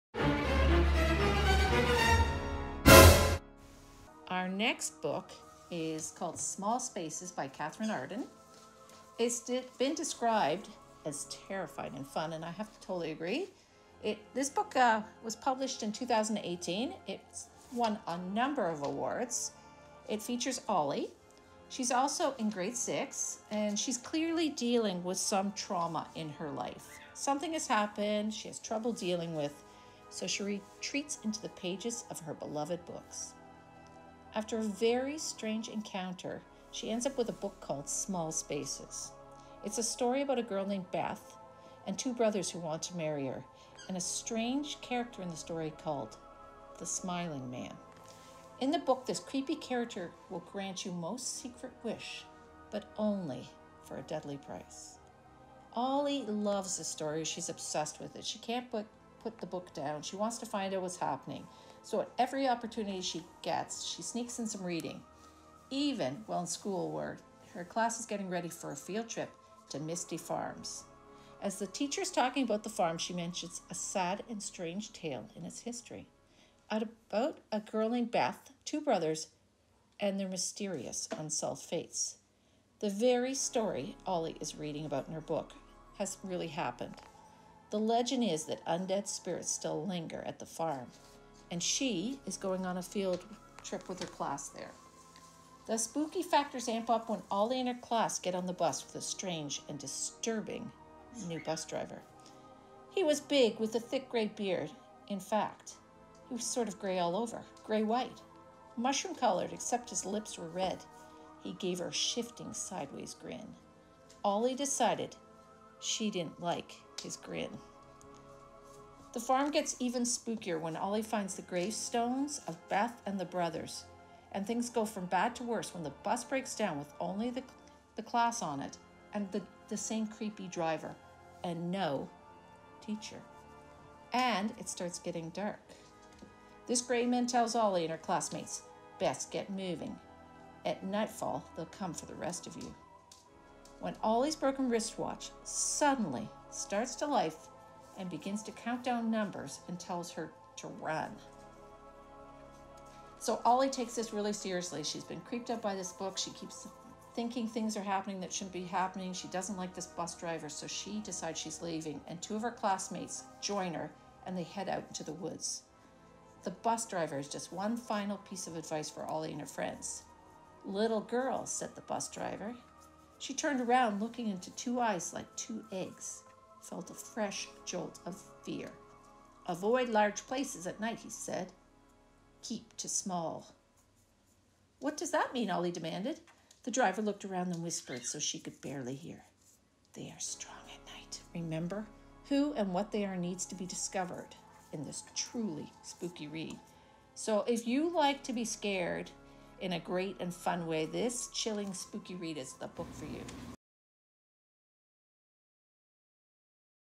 "Small Spaces" by Katherine Arden | Book Talk
Hear all about "Small Spaces" by Katherine Arden and find it at Kitchener Public Library

If you're looking for Young Adult novels in any genre, check out our Teen Reads tags